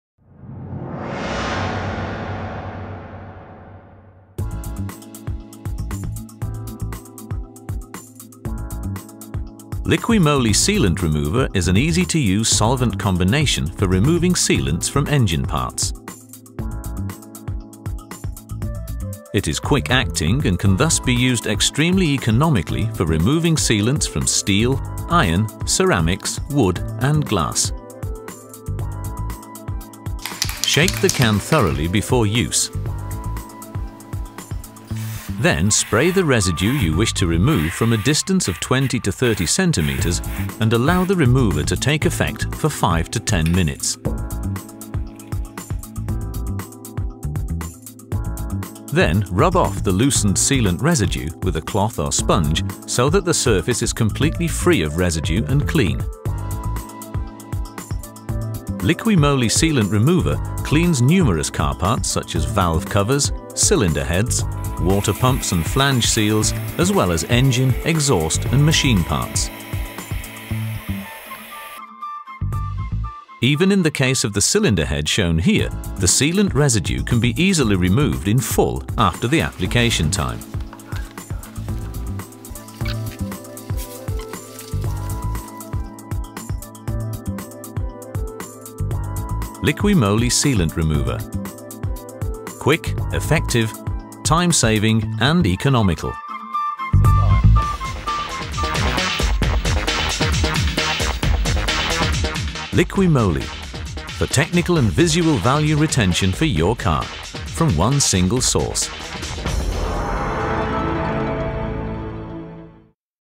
LIQUI MOLY Sealant Remover
Засоры, пригоревшие герметики, въевшиеся клея на двигателях и механических частях удаляются быстро и легко. Достаточно просто распылить средство. Продукт не стекает с вертикальных поверхностей.